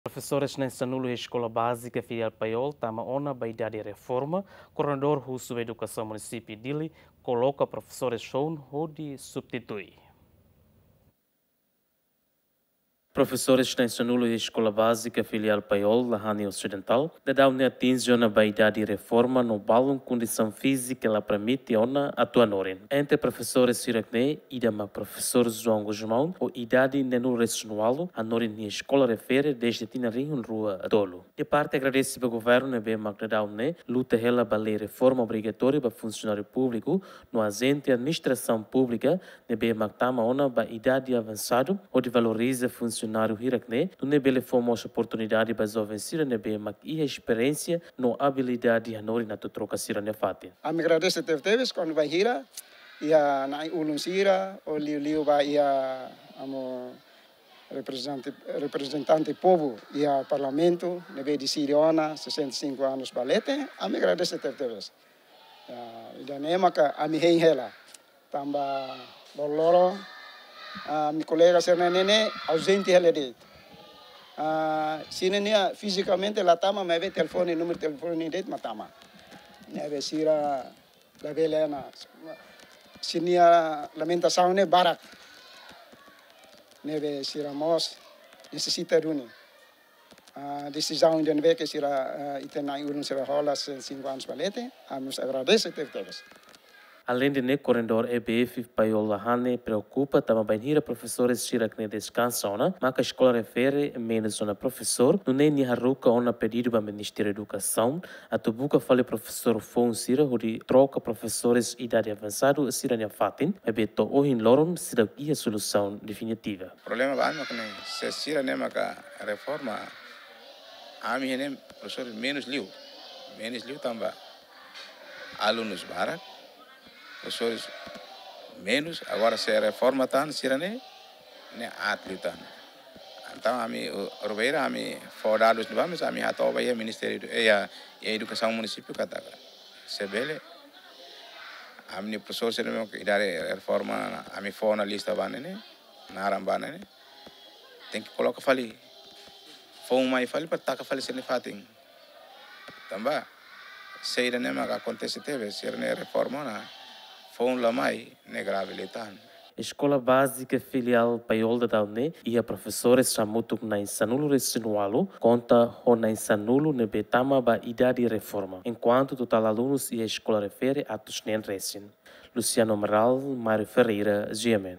Professores Nain 10 iha EBF Paiol tama ona ba idade reforma kordenador husu atu koloka...
Professores nain 10 iha Eskola Bázika Filial Paiol tama ona ba idade reforma Kordenador Husu Edukasaun Munisípiu Díli koloka Professores foun hodi substitui